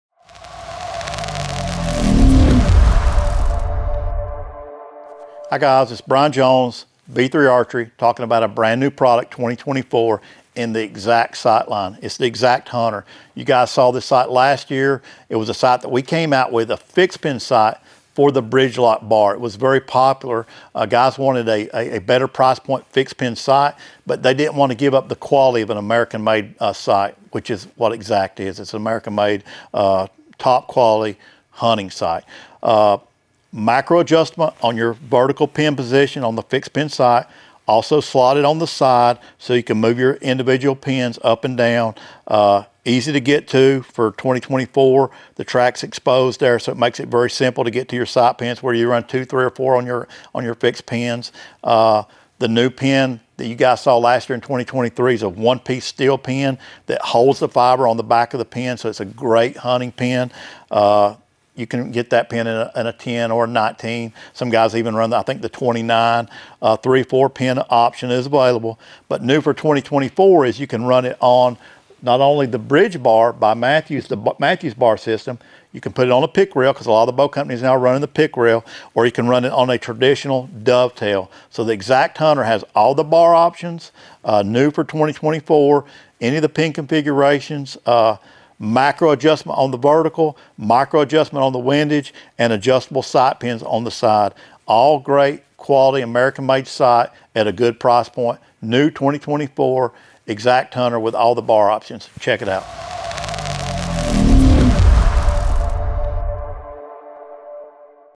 B3 ARCHERY - 2024 EXACT HUNTER SIGHTS
Checkout our latest upgrades to the Exact Hunter sight! A top quality fixed pin hunting sight now with the ability to mount it to any bow riser.

B3 Archery - Built the Best. By the Best. For the Best.

Follow along with us on social media! We would love to hear from you all!
Be sure to tag us @b3archery and use b3archery to share your photos and videos with us!